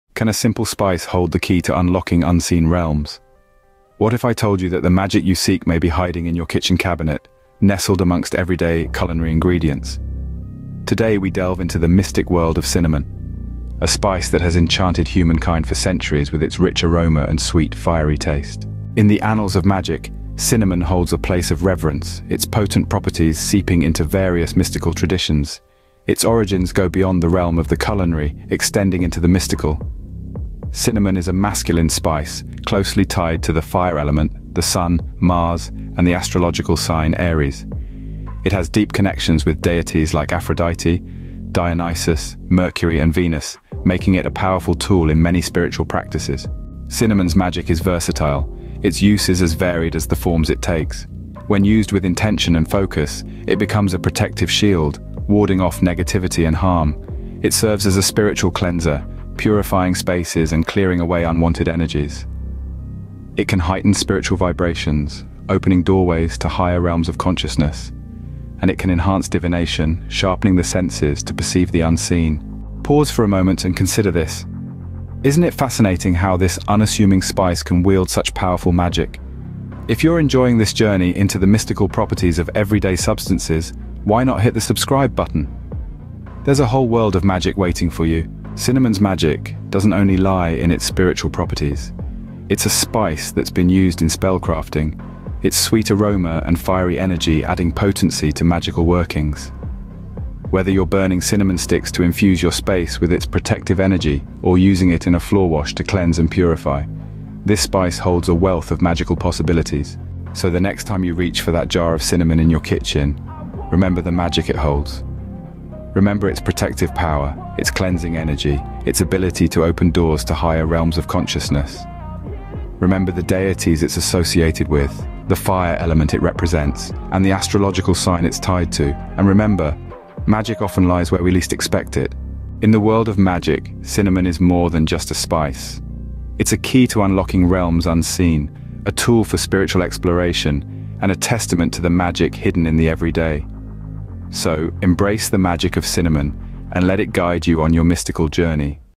Cinnamon Magick: Unveiling the Spice's Mystical Powers
Get ready to dive into the enchanting world of cinnamon! In this captivating video, we explore the magical properties of this seemingly ordinary spice and uncover its extraordinary powers. Join us as we delve into the realms of protection, divination, and spell crafting, and unlock the secrets of cinnamon's mystical abilities. From ancient rituals to modern practices, you'll be amazed at all the ways cinnamon can enhance your spiritual journey. Let's spread the magic!

OUTLINE:

00:00:00 The Mystical World of Cinnamon
00:00:18 Cinnamon's Mystical Origins
00:00:46 The Versatile Magic of Cinnamon
00:01:12 The Fascination of Cinnamon's Magic
00:01:28 Cinnamon in Spell Crafting
00:01:50 Embracing the Magic of Cinnamon
00:02:12 The Key to Mystical Realms